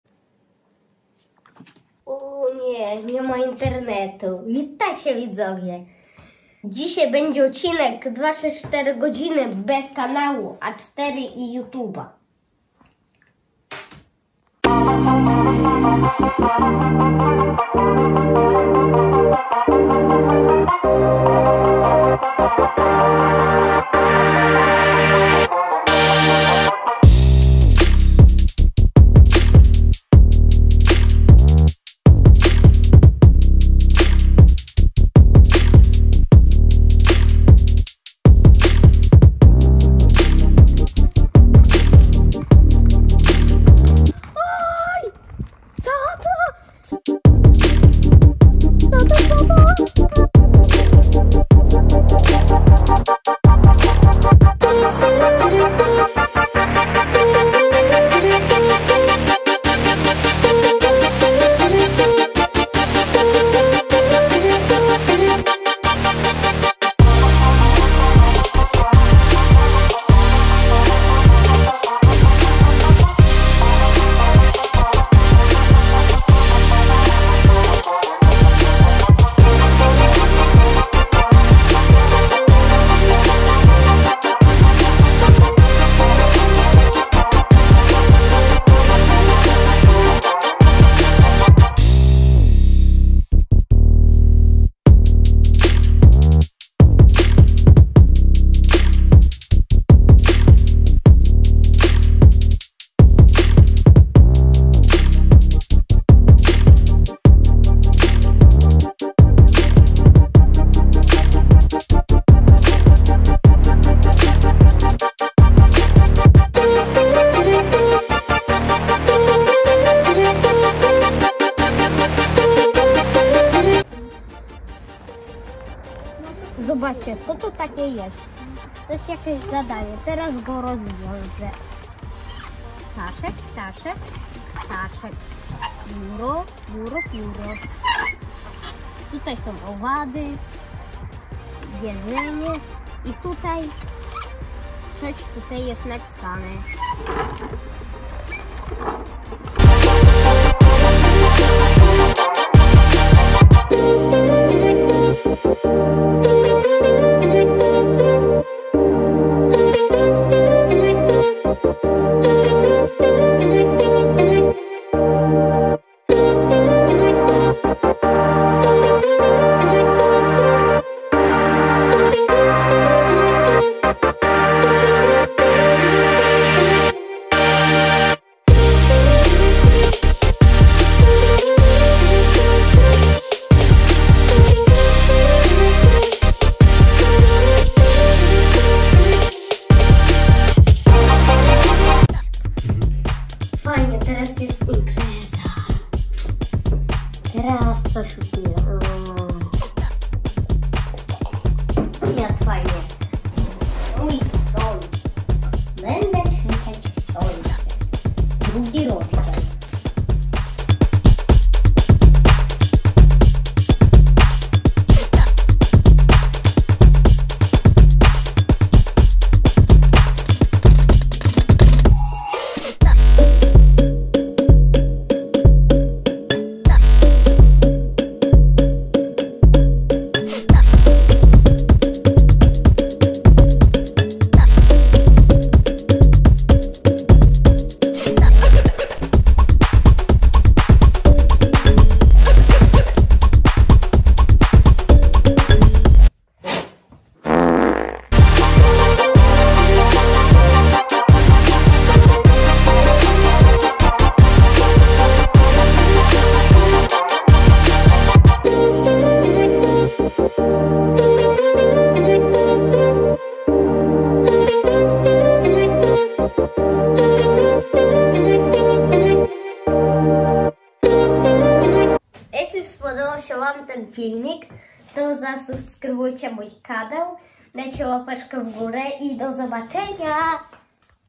24 godziny nie oglądam YouTube kanał A4
24 godziny nie oglądam YouTube kanał A4.  Nie mam internetu i 24 godziny nie oglądam kanał a4 i youtube . Pierwsze co robię to zbieram ziemniaki z pop it i wjechałem w jajo, później jadę do innego kraju, bardzo mi się spodobało w leśnym parku, oczywiście zjadłem kanapkę z serem, żeby być fajnym chłopakiem czytam książkę, kiedy już był wieczór zrobiłem imprezę, żeby być zdrowym umyłem zęby, później poszedłem spać bardzo śmiesznie, innego dnia kiedy wstałem zobaczyłem krew. Wszystko było w porządku bo to był flamaster.